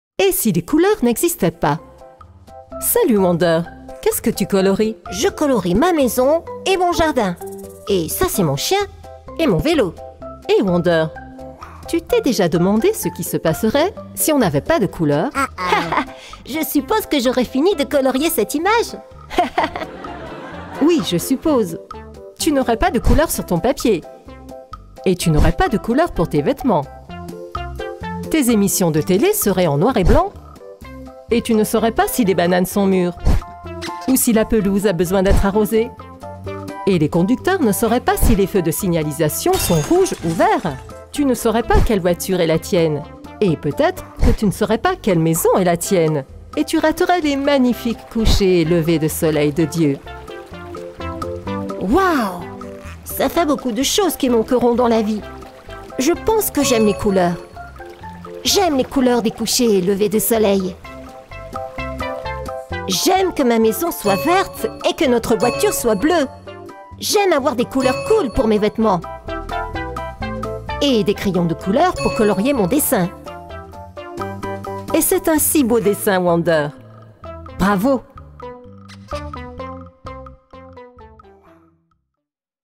Et si les couleurs n'existaient pas? Monde Sans Couleurs | Enfants Apprenant Des Histoires
Et si le soleil n'existait pas? L'importance des couleurs | Histoire en Français | Enfants Apprenant Des Histoires